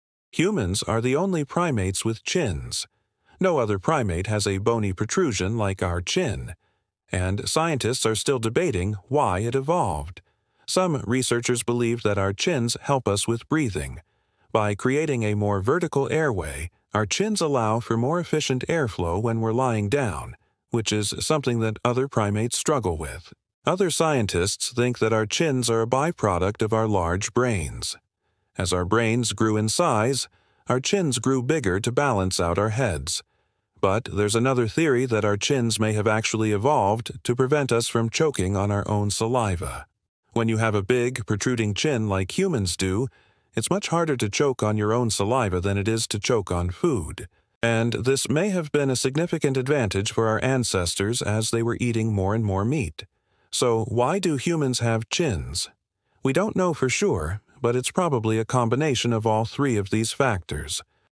Why Do ONLY Humans Have Chins? The Evolutionary Mystery Unveiled!
Humans are the only primates with a bony chin, and scientists are still debating why this unique trait evolved. Was it for speech, chewing, or even attractiveness? Dive into the fascinating theories behind this evolutionary puzzle and explore what sets us apart from other primates. This video unpacks the science, history, and mystery of the human chin in an engaging cinematic style.